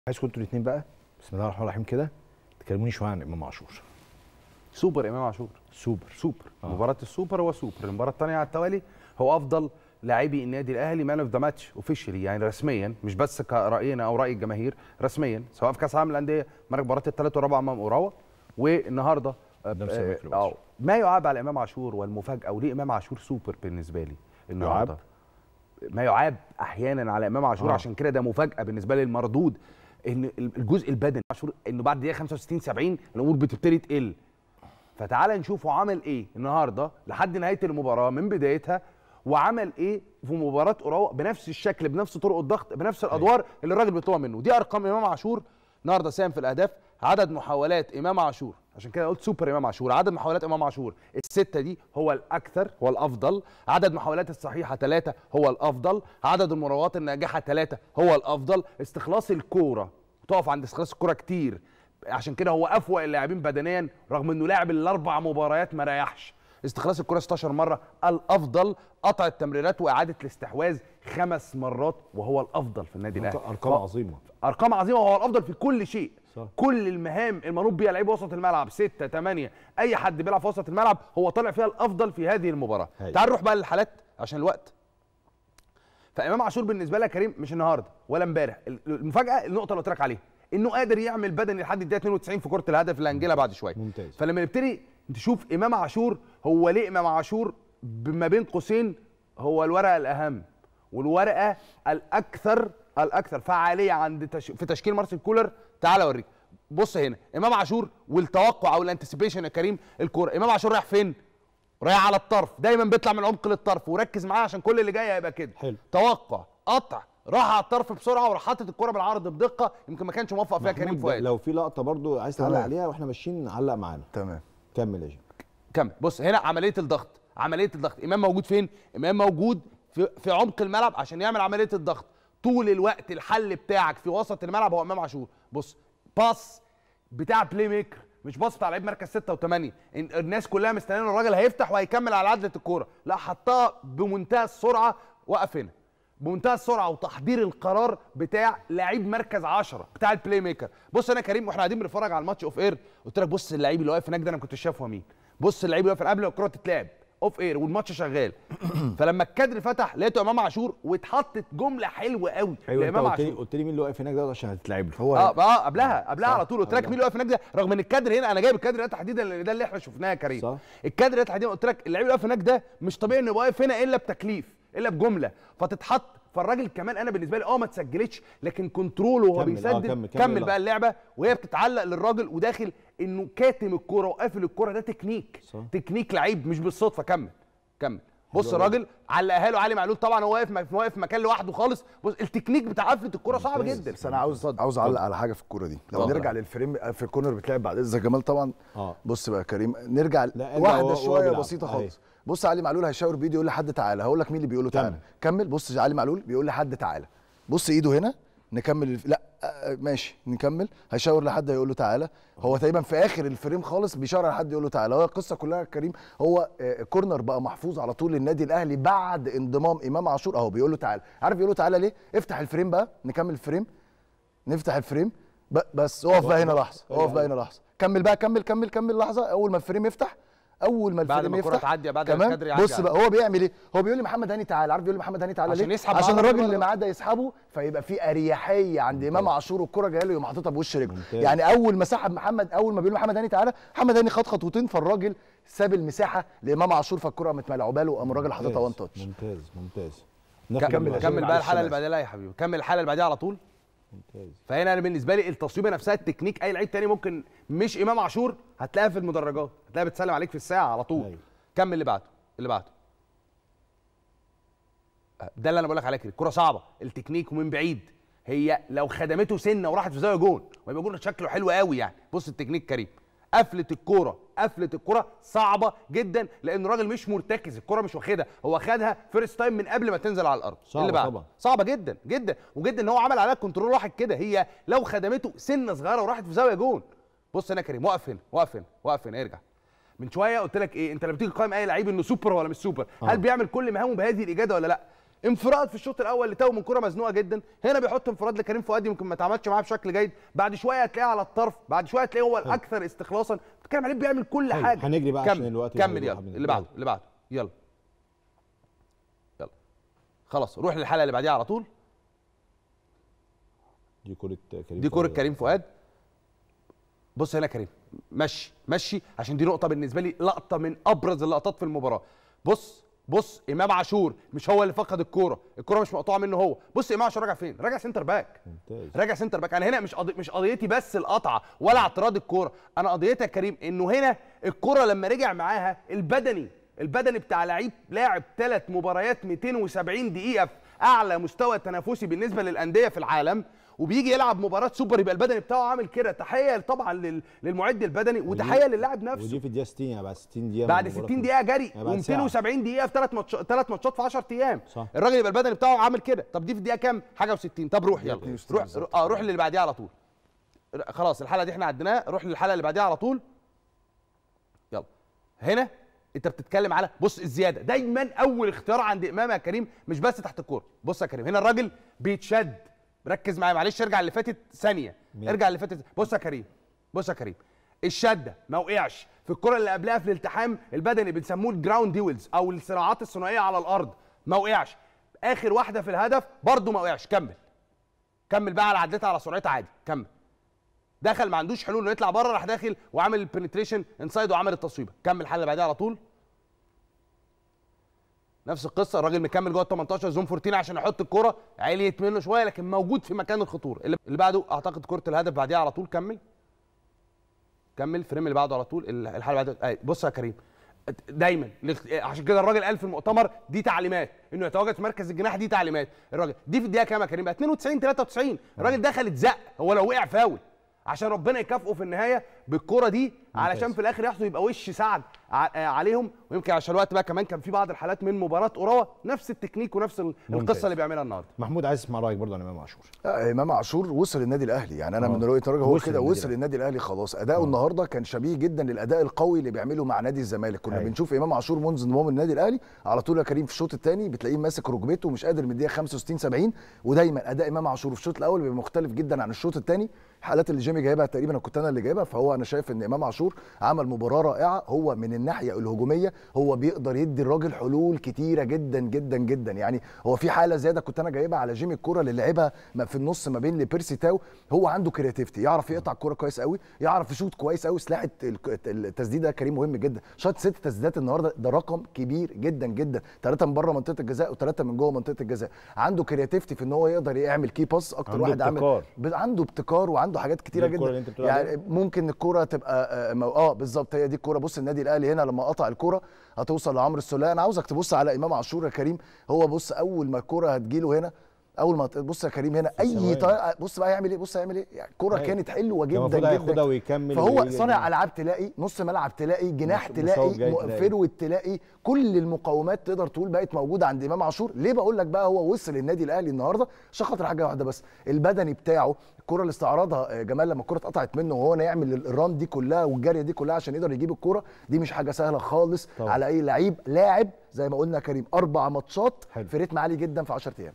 سوبر امام عاشور 💪⚽ .. محمد جمال يتحدث عن أرقام استثنائية لامام عاشور مع الاهلي 🔥💣
سوبر امام عاشور 💪⚽ .. محمد جمال يتحدث عن أرقام استثنائية لامام عاشور مع الاهلي 🔥💣 ومحمود شاكر يكشف عن تكنيك مختلف للأهلي في الركنيات استخدمه أمام سيراميكا🔥💣🔴👇🏻